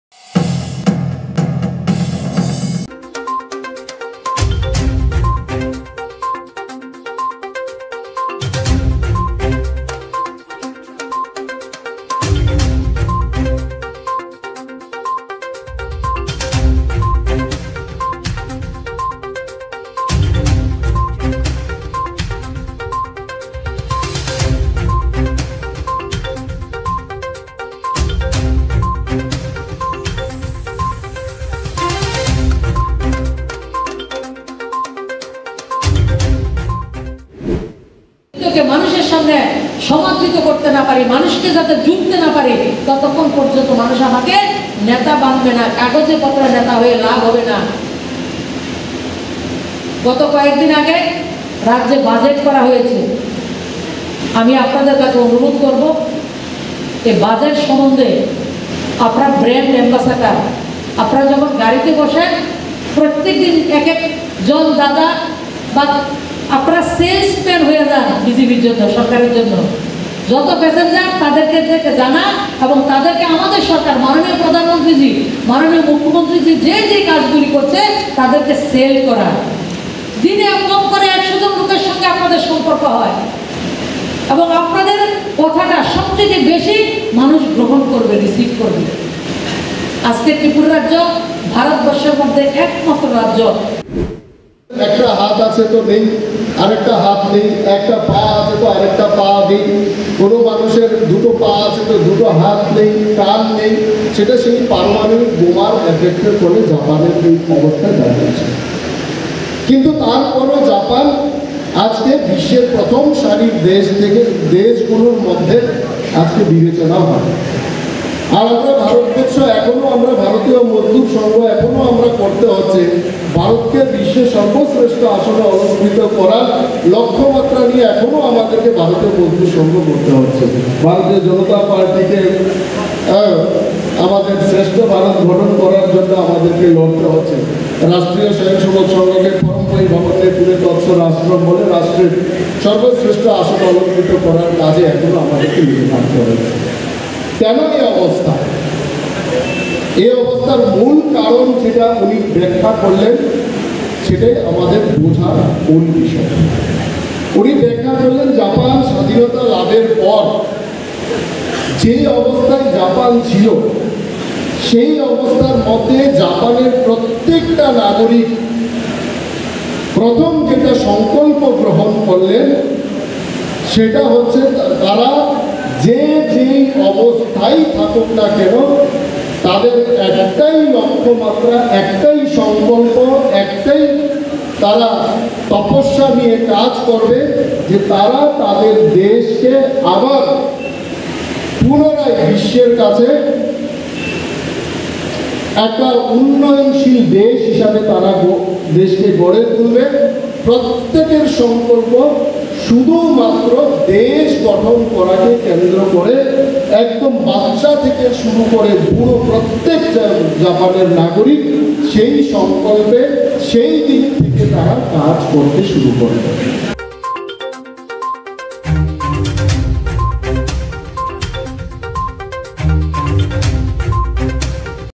ভারতীয় মজদুর সংঘের সিপাহীজলা ত্রি-বার্ষিক জেলা সম্মেলন অনুষ্ঠিত || VOICE 18
VOICE 18
Name of Production House: FLAXEN MEDIA WORKS

বিশালগড় থেকে  জয়দেব দেবনাথ এর রিপোর্ট, VOICE 18